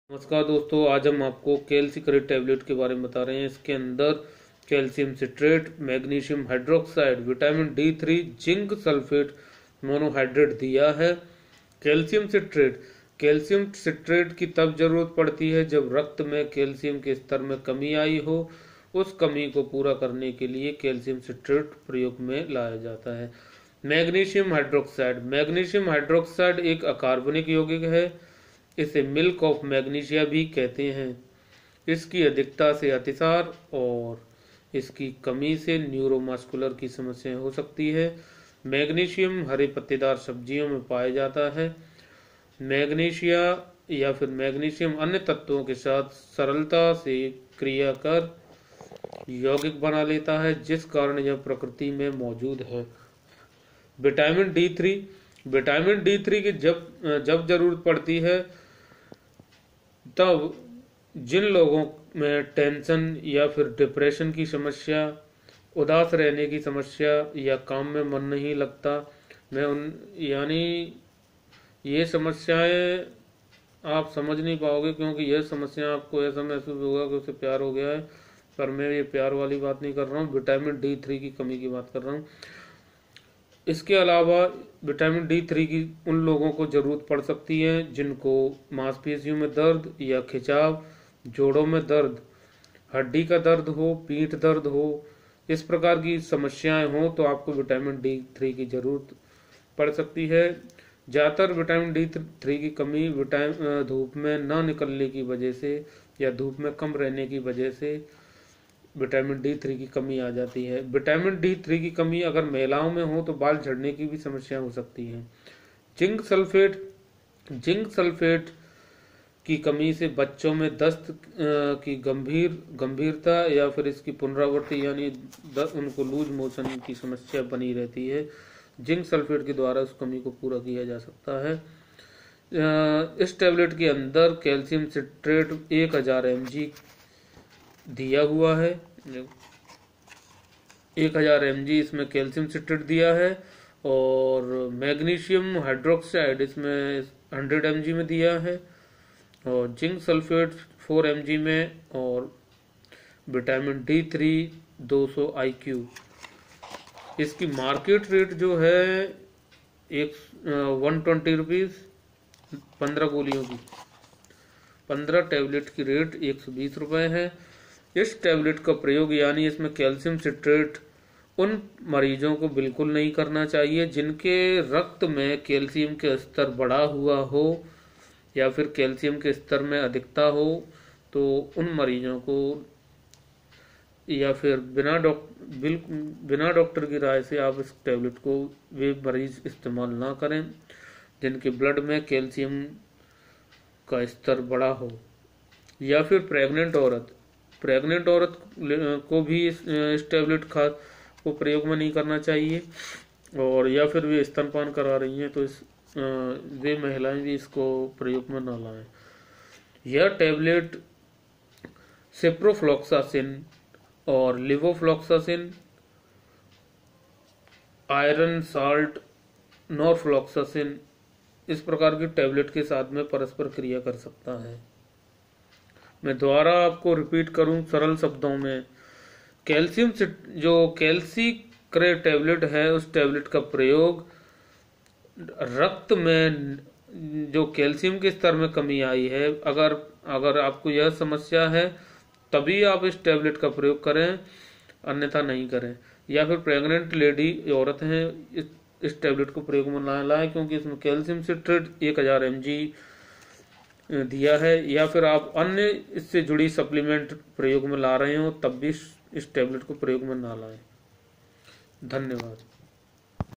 CALCI - KARE Tablet(Calcium citrate,Megnesium, vit.D3,Zinc sulphate )
Calci -kare Tablet
            रक्त में calcium के स्तर को बढ़ाने के लिये इस टेबलेट को प्रयोग में लाते हैं ।
Megnesium hydroxide :.
         एक अकार्बनिक योगिक है इसे मिल्क ऑफ मैग्नीशिया कहते हैं । इसकी अधिकता से अतिसार और कमी से न्यूरोमास्कुलर समस्या हो सकती है ।
  vitamin D3 :.
                अगर आप की हड्डियों में दर्द , पीठ दर्द , जोड़ों में दर्द , अगर आप टेन्सन , डिप्रेसन की समस्या , उदास रहना , काम में मन न लगना आदि समस्या हो तो vitamin D3 की जरूरत हो सकती है ।
        अगर जिनके रक्त में calcium की मात्रा अधिक हो तो उन्हें इस टेबलेट को प्रयोग नहीं करना चाहिये , गर्भवती को , स्तनपान कराने वाली महिलाओं को इस टैबलेट को प्रयोग नहीं करना चाहिये ।